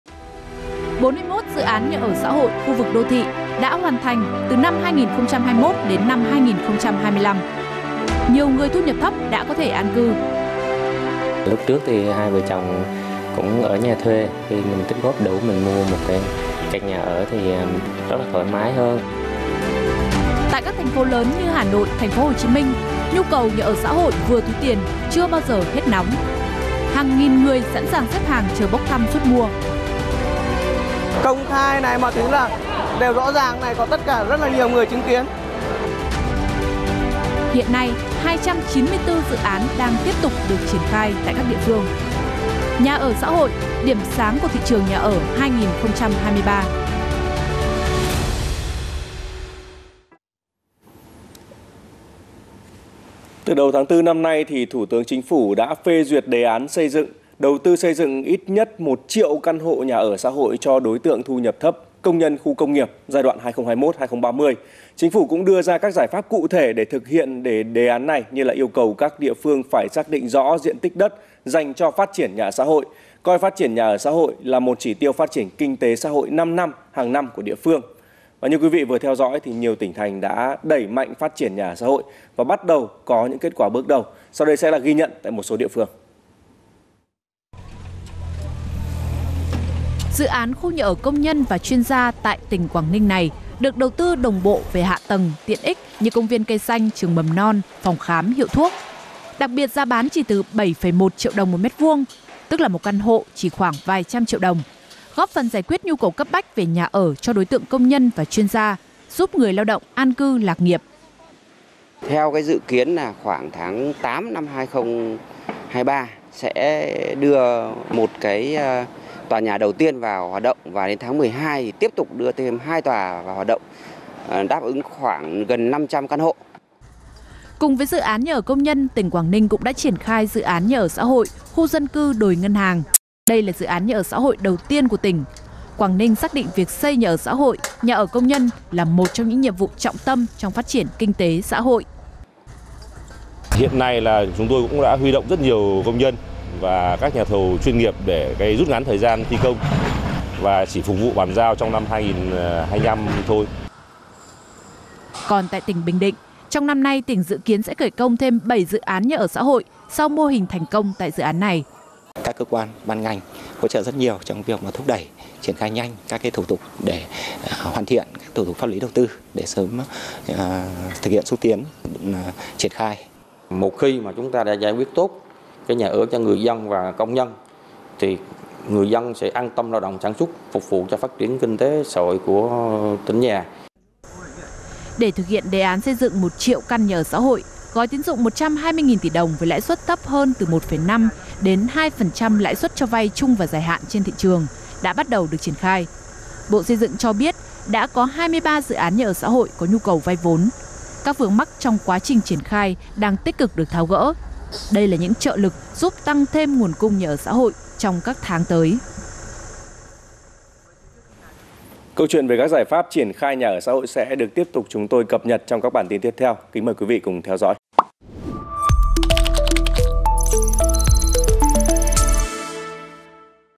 Nhà ở Xã hội- Điểm sáng 2023 | VTVMoney
41 dự án nhà ở xã hội khu vực đô thị đã hoàn thành từ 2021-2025.
Nhiều người thu nhập thấp đã có thể an cư,